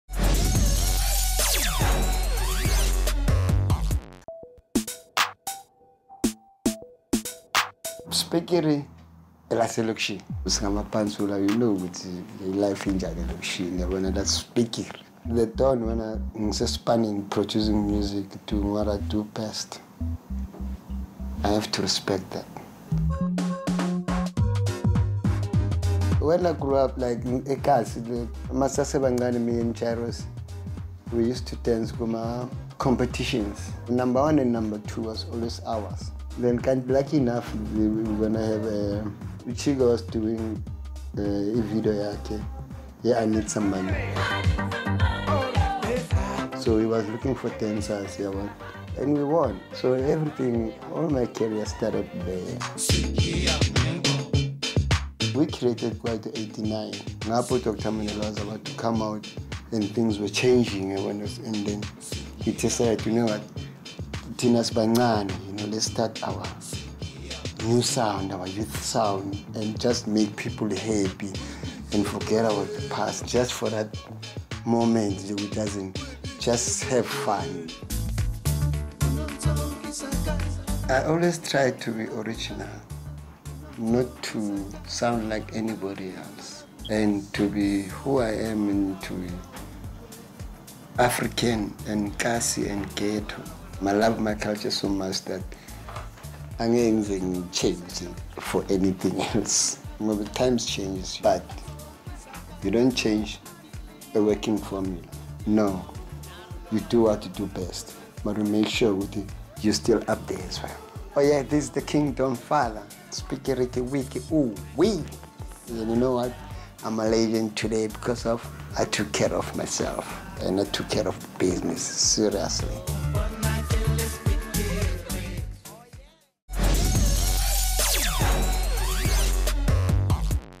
Spikiri recalls the pantsula beginnings! | V-Entertainment
Kwaito legend and former Trompies member Spikiri chatted to us about the genesis of the icon group, his former pantsula dancing days and more in this exclusive interview